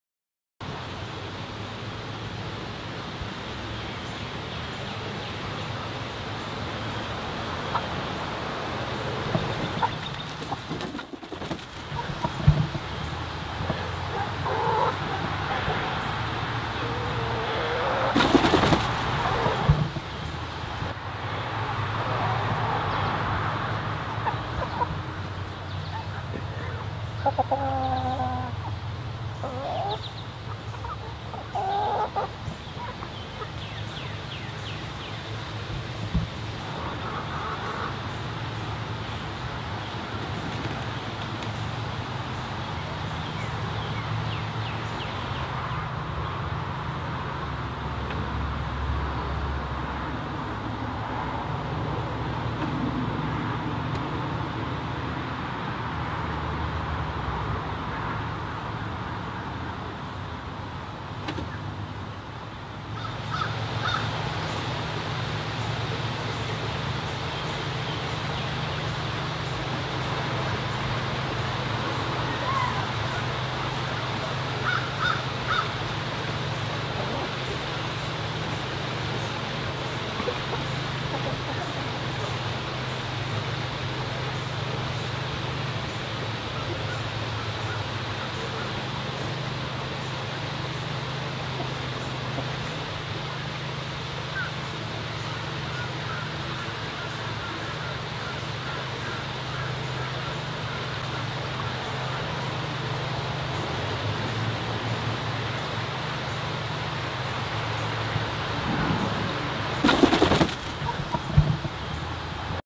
Those Crazy Chickens
The chickens on a recent morning..i'll be missing them...no doubt.